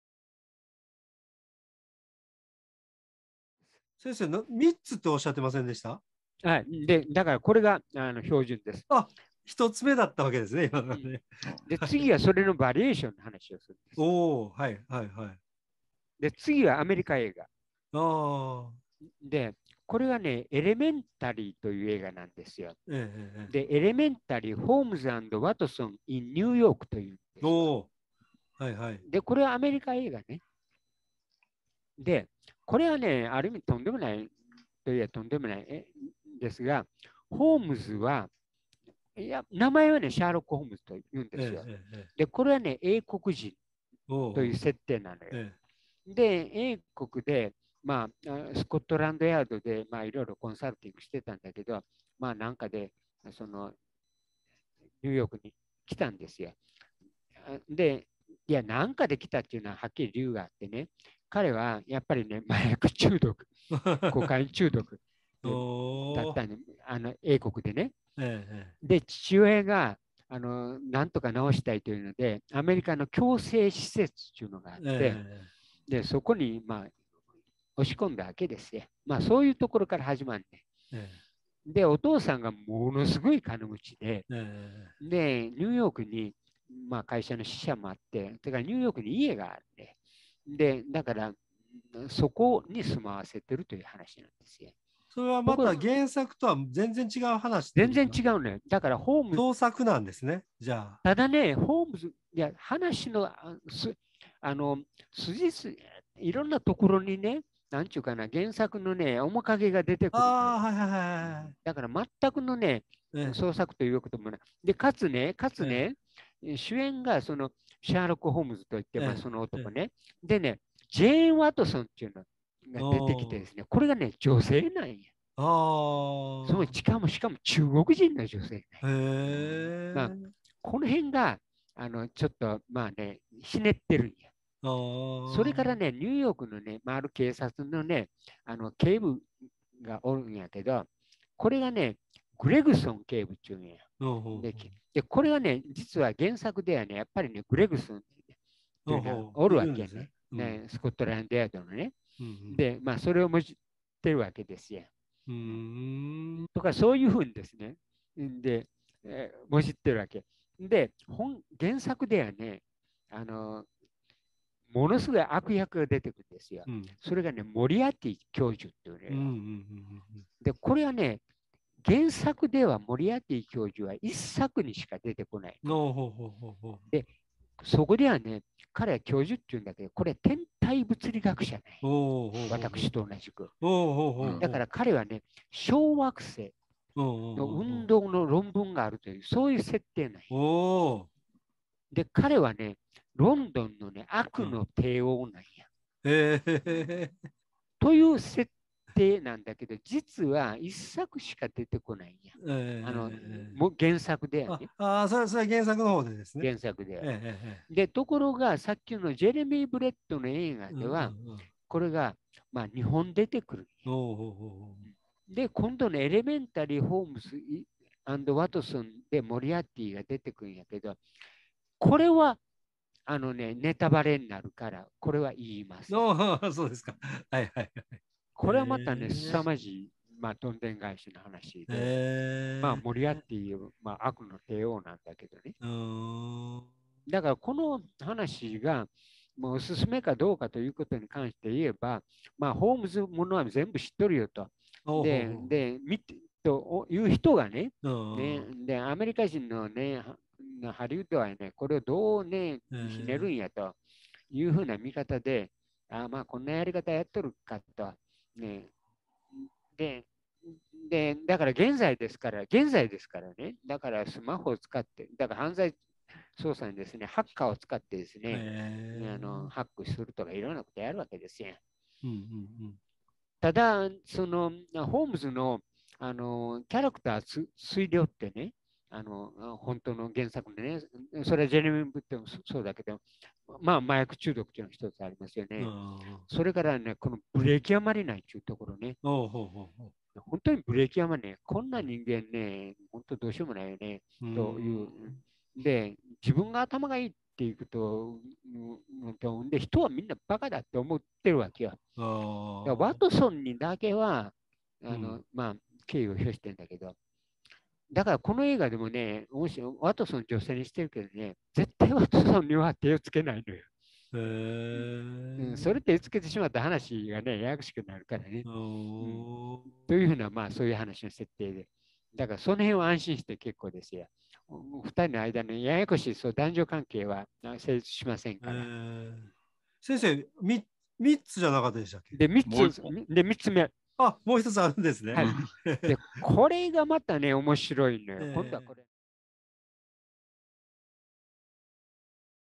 松田語録：夏休み質問コーナー②夏休みにおすすめの映画・ドラマは？（その３）エレメンタリー ホームズ&ワトソン in NY （②ー２－２）
シンギュラリティサロン主宰の松田卓也神戸大学名誉教授の健康や学習に関連する日ごろのお考えを皆さんにお伝えします。今回は「夏休み質問コーナー」と称して松田先生に何でも聞いてみようかと思い、10の質問を用意しました。
①宇宙人はいるのか？
②夏休みにおすすめの映画・ドラマは？
③人は死んだらどうなるのか？
④宇宙の果てはどうなっているのか？
⑤コロナは今後どうなるのか？
⑥近い将来戦争は起こるか？
⑦もっとおいしい食べ物は現れるか？
⑧東海大地震は起こるか？
⑨バーチャル空間で人類は何をすべきか？
⑩タイムマシンは実現可能か？

今回は、②夏休みにおすすめの映画・ドラマは？に対する２つ目の答え。
「シャーロックホームズいろいろ」とのことで、これからシャーロックホームズが
続きます。その２つめは次のテレビドラマ（つまり②ー２－２）。

アメリカTVドラマ
エレメンタリー　ホームズ＆ワトソン in NY
『エレメンタリー ホームズ&ワトソン in NY』シーズン１
吹き替え版
※シーズン７まであります

語り手：松田卓也　シンギュラリティサロン主宰・神戸大学名誉教授
聞き手：塚本昌彦　神戸大学教授、小林秀章　セーラー服おじさん